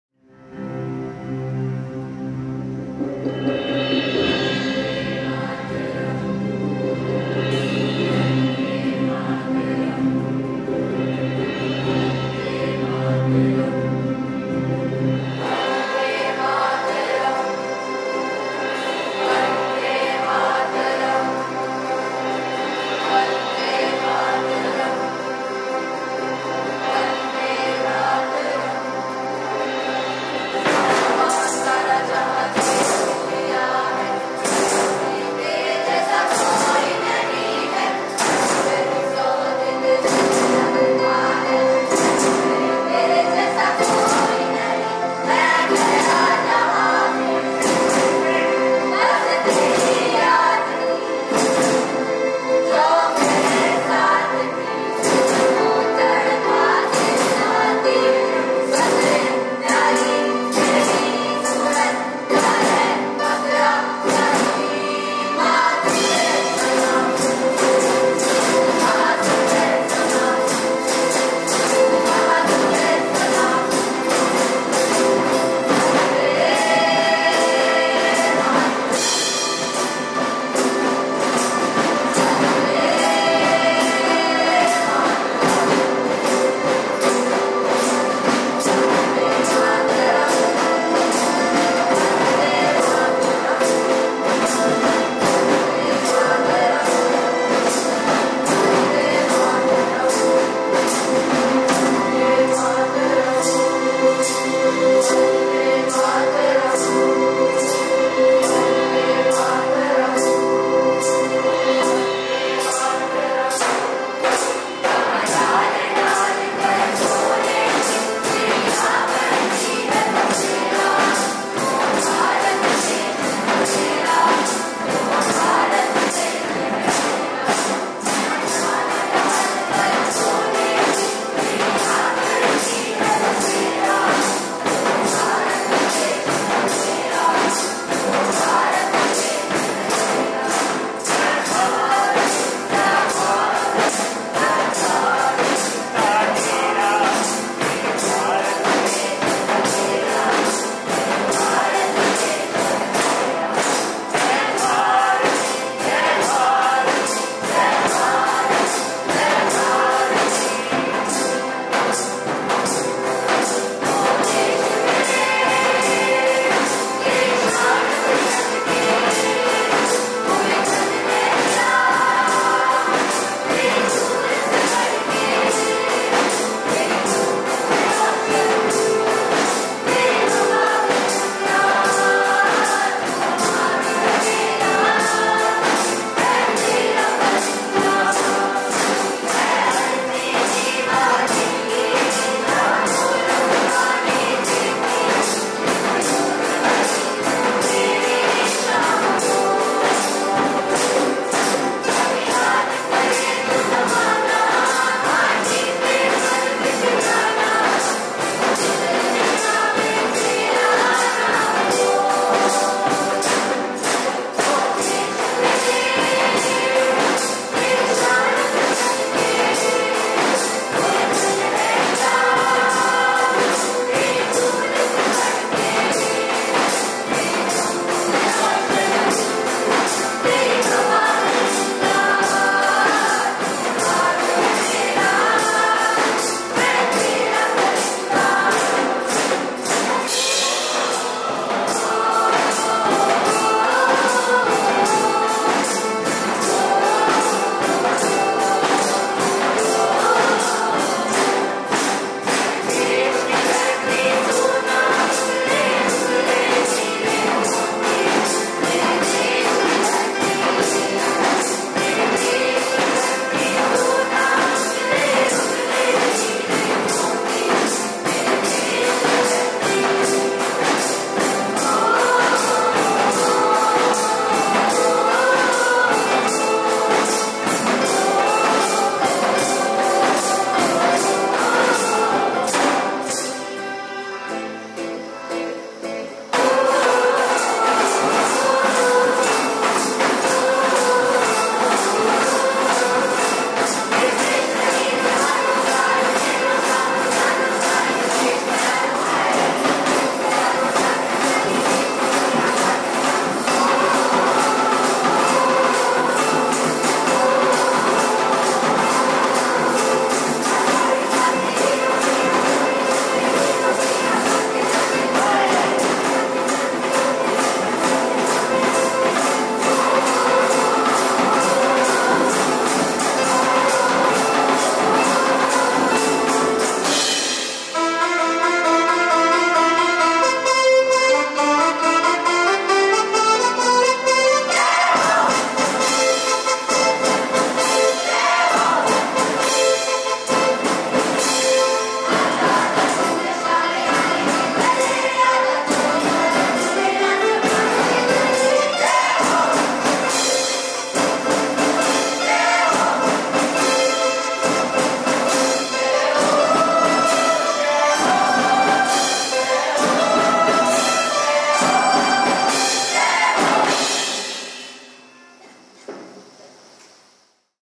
Student led Independence Day celebration at Titiksha Public School | Happy Independence Day 2023
Wishing all the students, teachers, and staff a joyous and proud Independence Day. May the spirit of freedom, unity, and progress continue to guide our nation's journey. Let's remember the sacrifices of our heroes and work together to build a brighter future for India. Jai Hind! 🙌🎉🌟